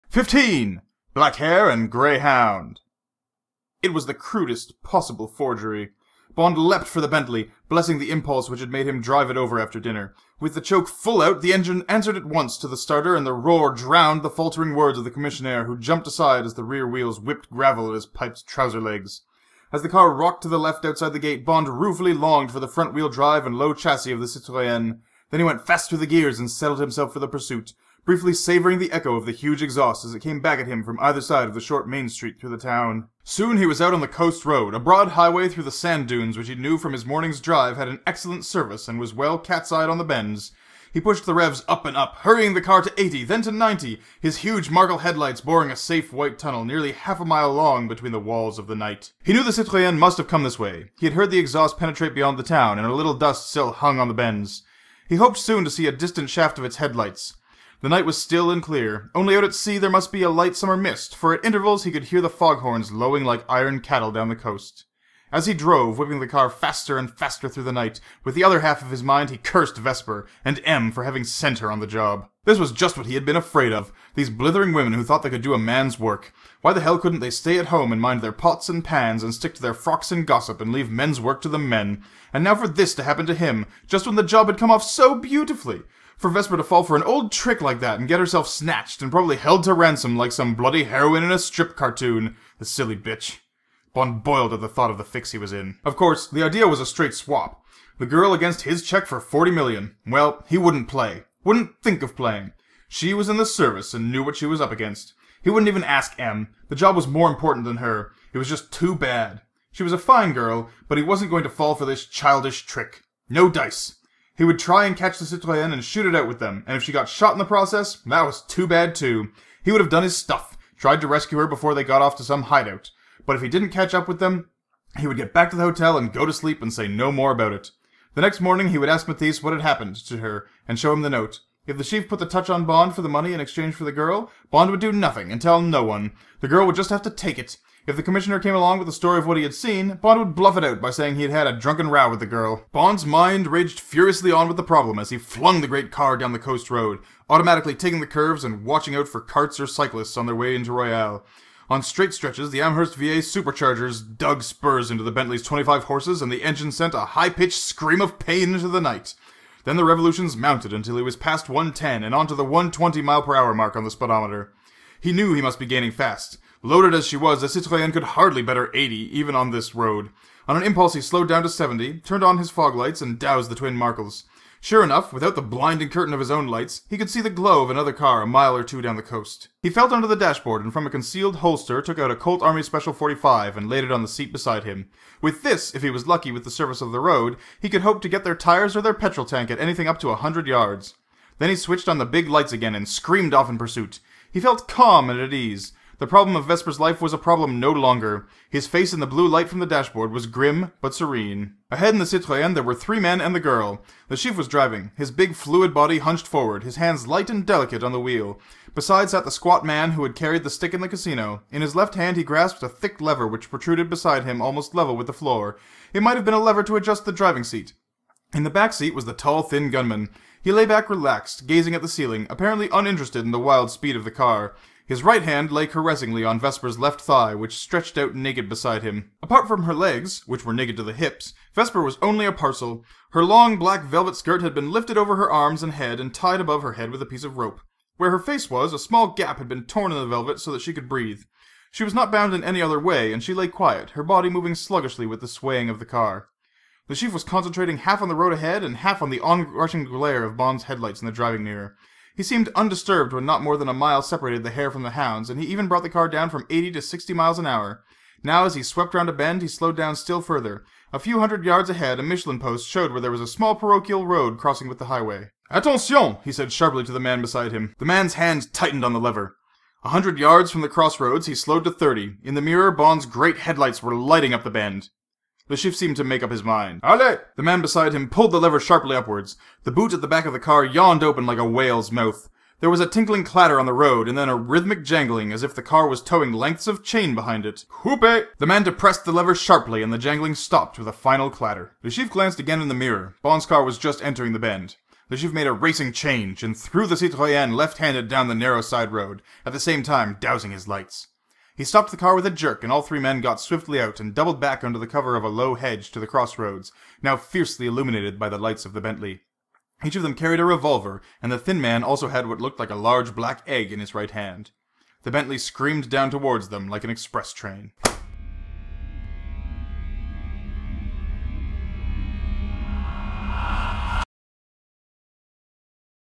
15 — Black Hare and Grey Hound
Our hero has some serious selfishness issues to work on.  As well as the misogyny we already knew about.  Through his rage, will be pay enough attention to notice the trap that he clearly does not expect?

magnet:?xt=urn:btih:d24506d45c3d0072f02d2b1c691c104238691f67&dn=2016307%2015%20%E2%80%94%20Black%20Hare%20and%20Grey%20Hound